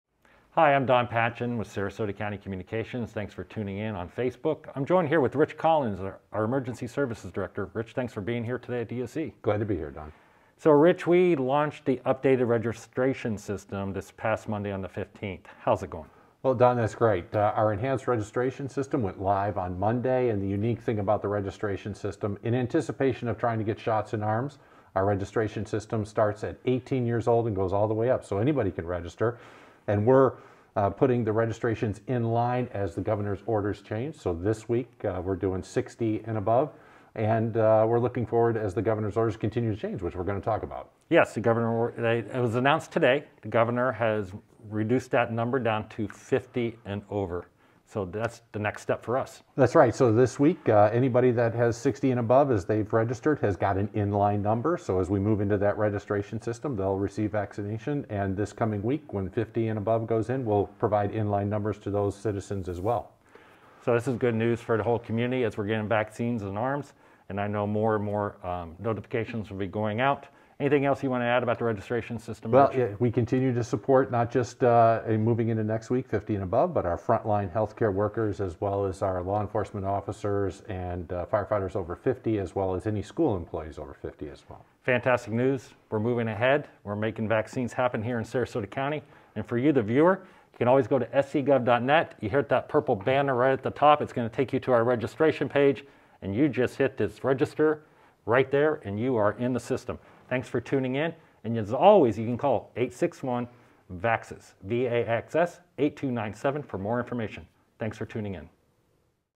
Sarasota County Vaccine Registration System Update: March 19, 2021
✅ Florida residents 18+ are eligible to register.
✅ 60+ Florida residents are being issued in-line numbers.
✅ Florida residents 50+ are eligible for vaccination starting Monday, March 22, per the Governor's latest executive order.
✅ The Department of Health in Sarasota County continues to work though vaccinating group 1A registrants, those 65+ and frontline healthcare workers.
✅ Visit scgov.net to register for your vaccine.